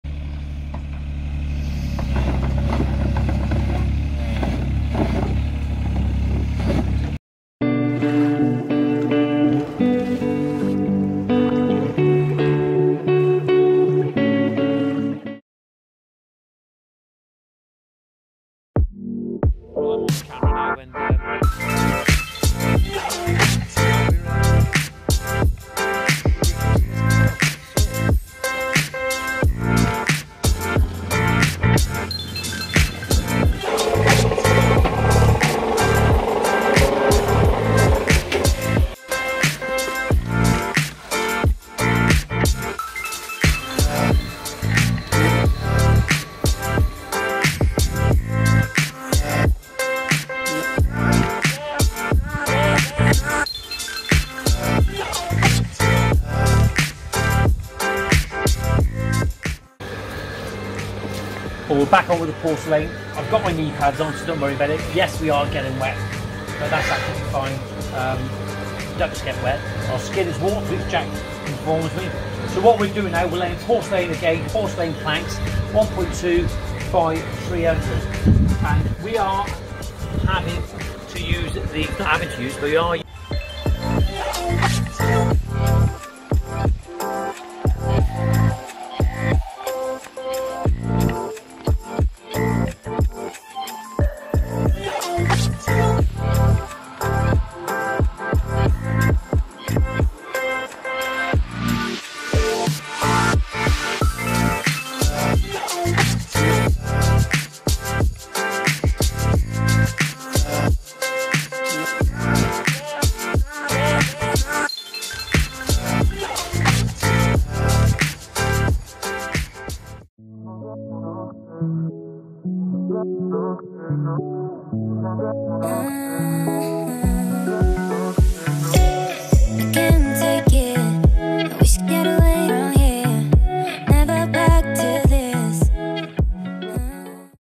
How To Lay Porcelain Patio The Correct Way
Porcelain Plank Patio Garden Transformation in this garden in Newport for Gareth and his Dad Mike who liaised with us, such nice people to work for.

Suppliers for the day